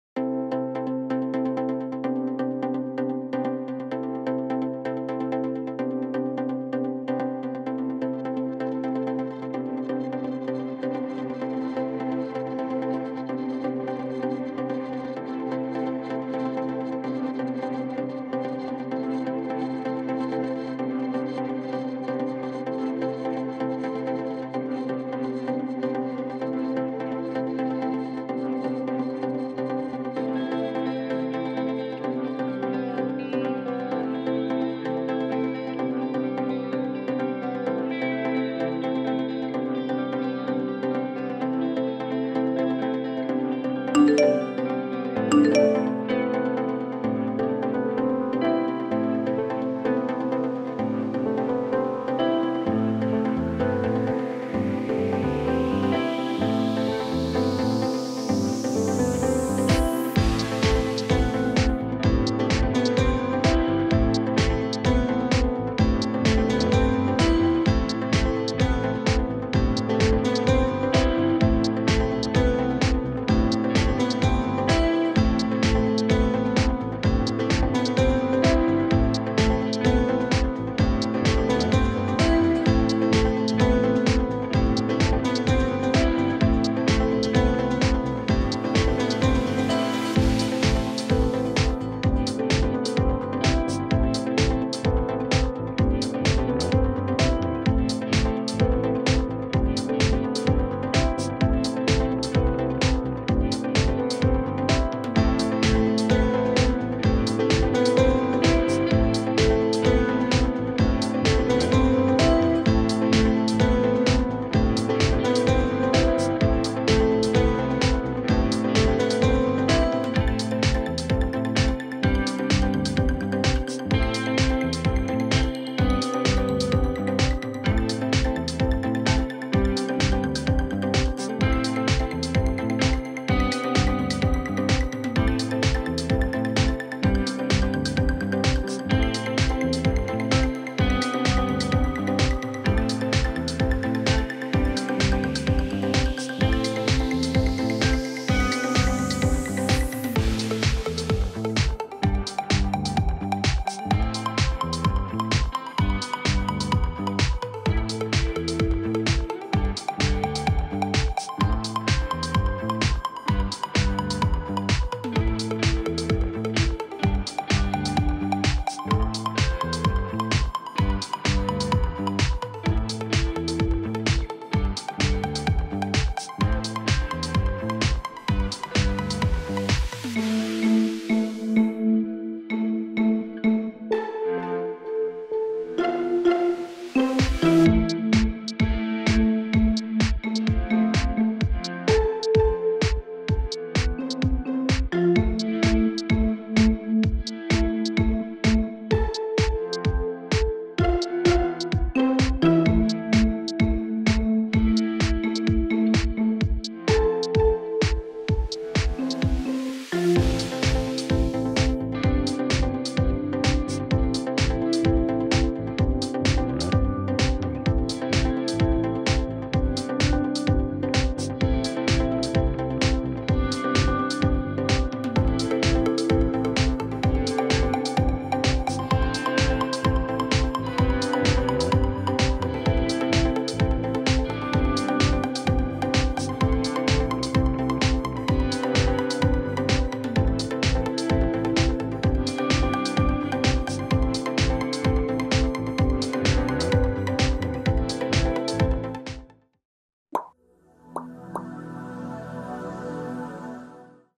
Hướng dẫn cách vẽ hình CON THỎ đơn giản #5 | How To Draw A Simple Rabbit
Chúng mình tiếp tục cùng nhau tập vẽ hình CON THỎ dễ thương - vẽ chủ đề ĐỘNG VẬT nhé!

Easy Rabbit Drawing Tutorial for Kid step by step.